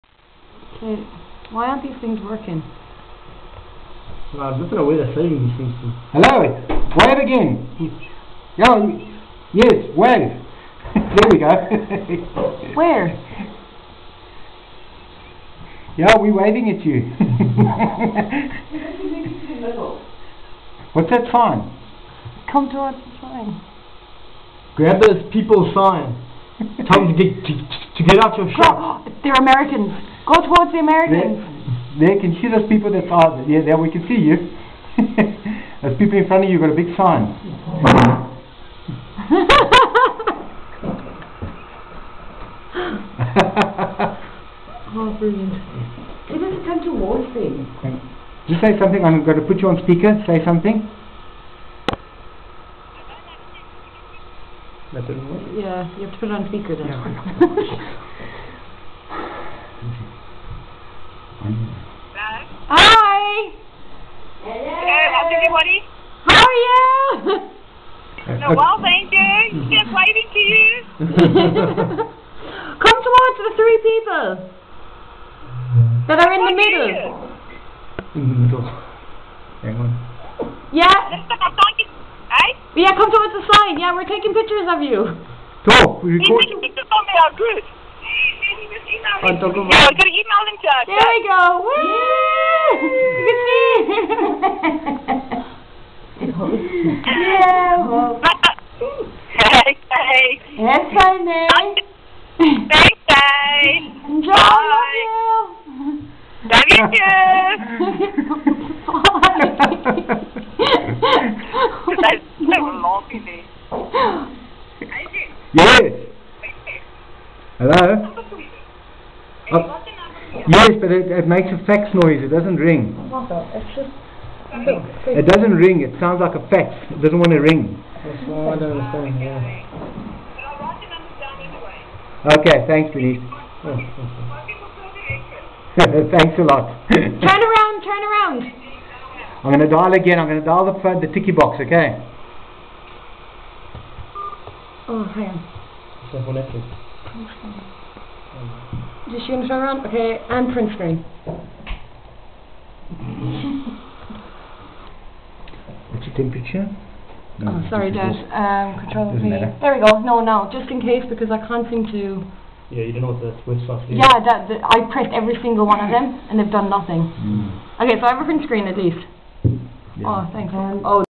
Times Square Live Featuring Bernice & Kathy
Watching Bernice & Kathy live from Nats laptop in Galway, Ireland